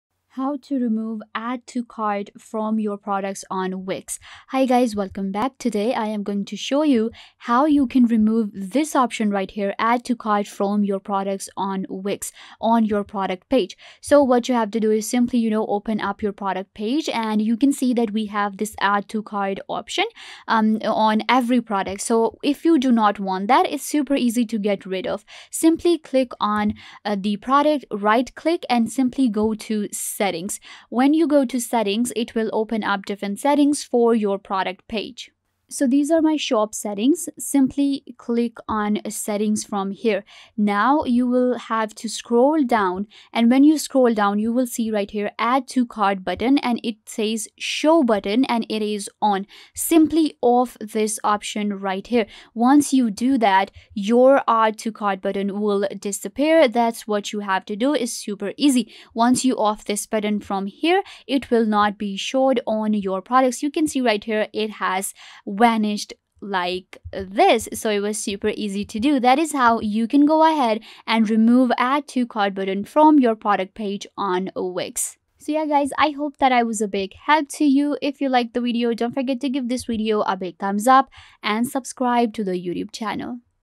How To Remove Add to Cart from Product Page on Wix | Full Guide
After watching this video you will know exactly how you can remove add to cart from product page on Wix within a few minutes.